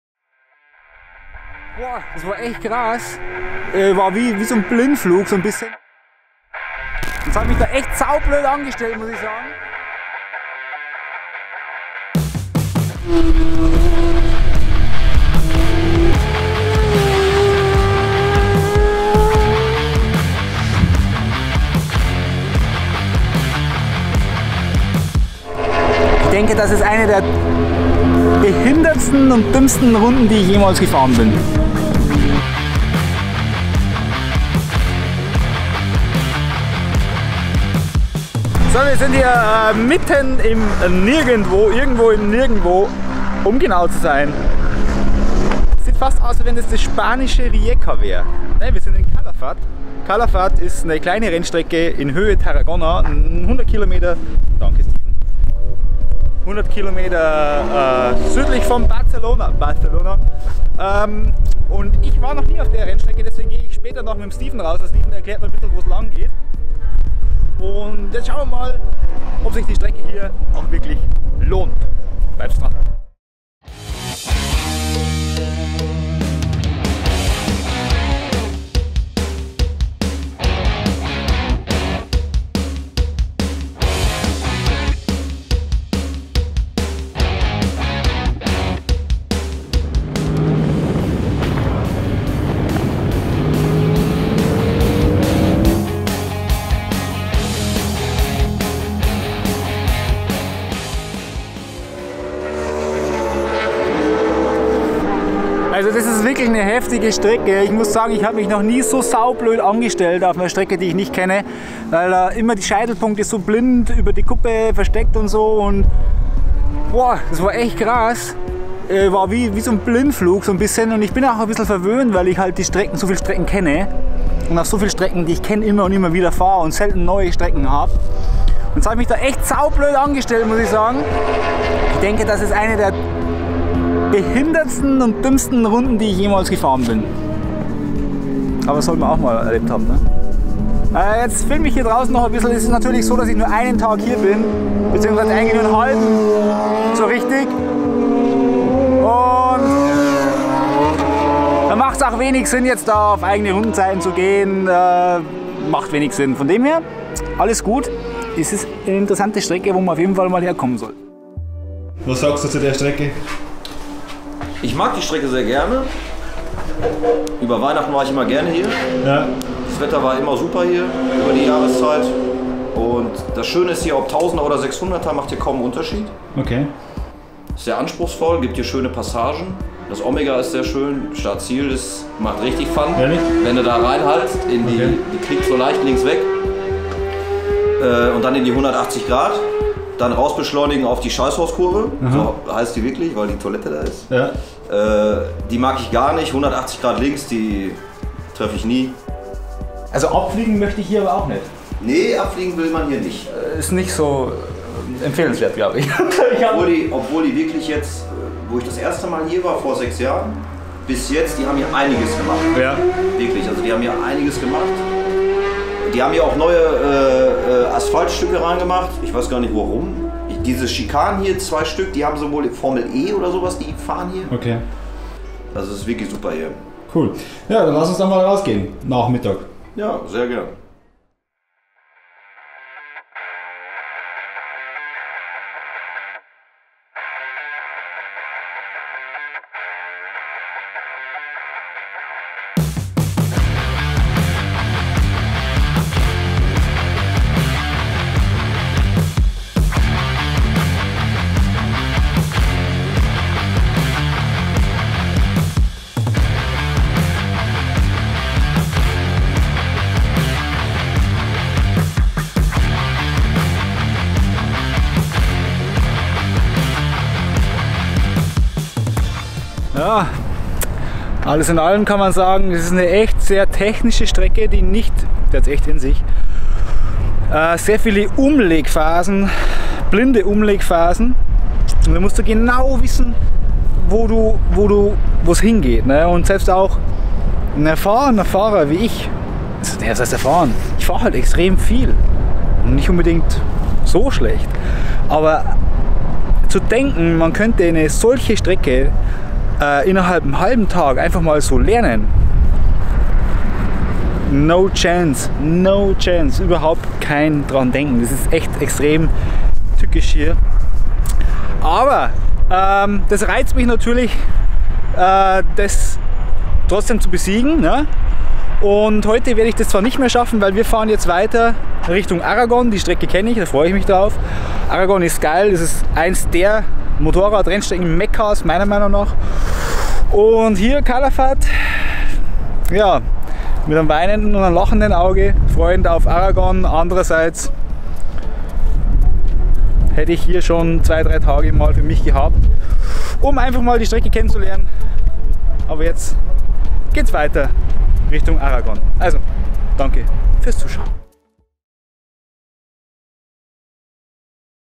Unmöglich an einem Tag zu lernen! | Du solltest dir mehr Zeit nehmen als ich!
Unmöglich an einem Tag zu lernen! | Du solltest dir mehr Zeitnehmern als ich!

Ich drehe meine ersten Runden in Calafat und habe durchaus meine Schwierigkeiten, mich auf diesem, für mich neuen Kurs, zurecht zu finden

Zu den Rennstrecken Events:
Für Einsteiger:

Die Blickführung!